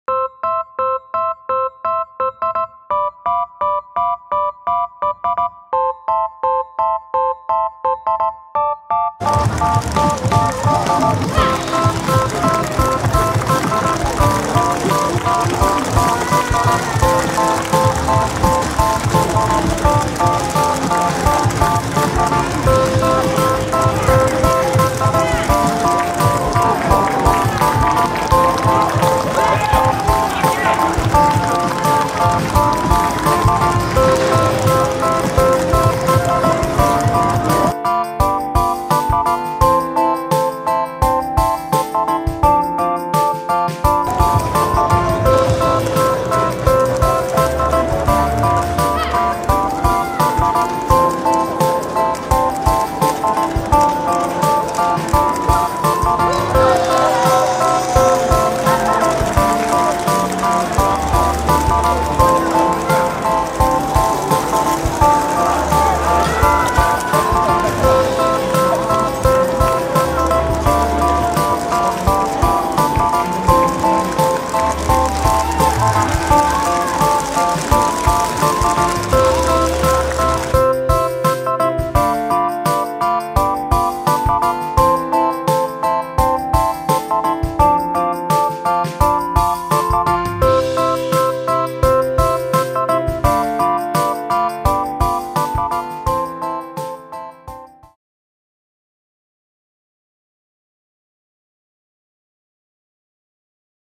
전국체육대회 장대높이뛰기 조민지
2022109 울산종합운동장에서 열린 '제103회 전국체육대회' 육상 장대높이뛰기 여자일반부 3.65M에 도전하는 조민지 경기 모습.